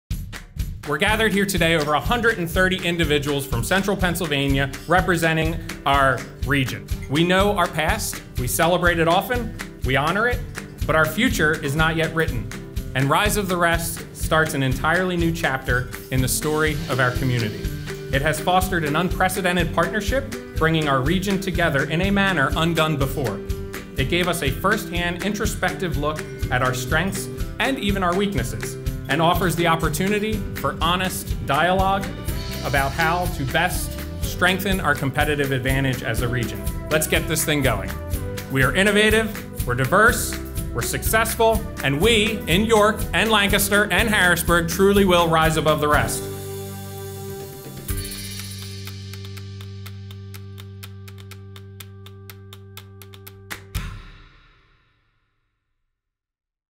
Rise of the Rest - 10/10/17
On 10/10/17, iconic investor and co-founder of AOL, Steve Case, and his Project Revolution team visited York, Lancaster and Harrisburg as the first stop of the Rise of the Rest 6.0 tour.

Thank you to Bold Creative Media for capturing the York portion of the "Central PA Rises" visit.